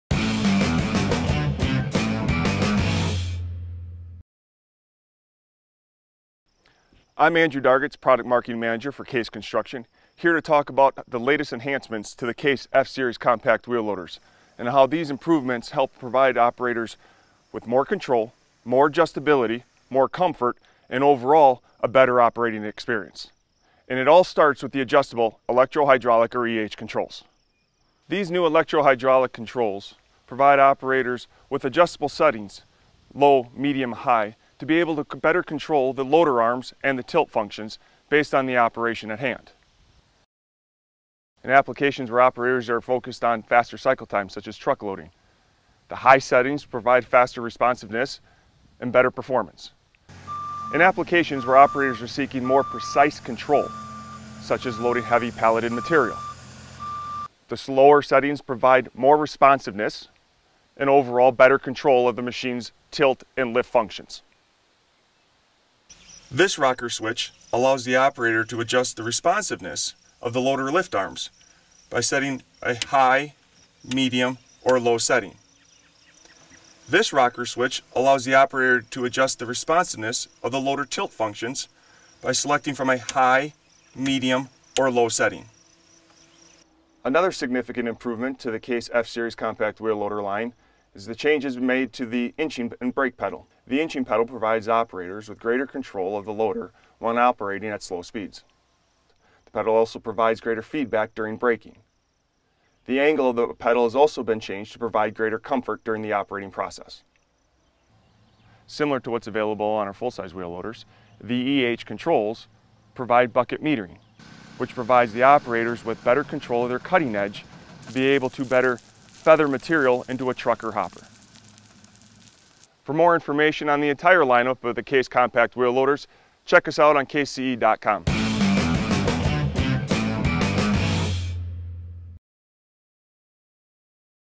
North America: TechTalk: New Compact Wheel Loader Electro-Hydraulic Controls
CASE has made it easier than ever to dial in compact wheel loader performance with all-new electro-hydraulic controls that allow operators to dial in boom and bucket speeds independently to match the demands of the job. Inching and bucket metering have also been improved in this latest update from CASE.

For more on the entire lineup of CASE compact wheel loaders,